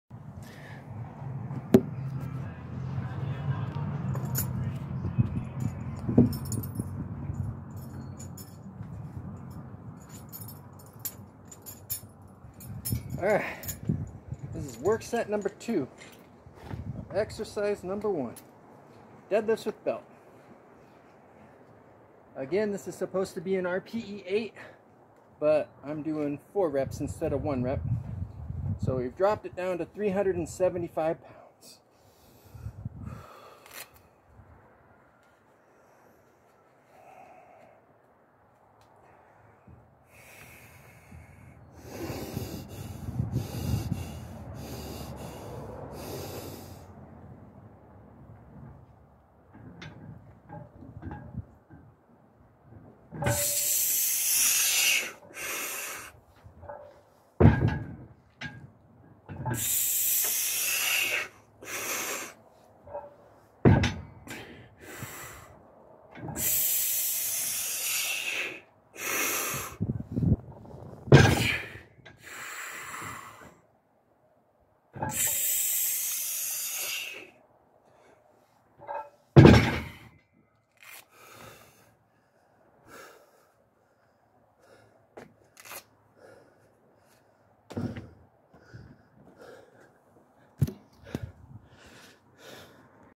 Deadlifts with beli 4x375
Work set 2 of exercise 1 of day 3 of week 6.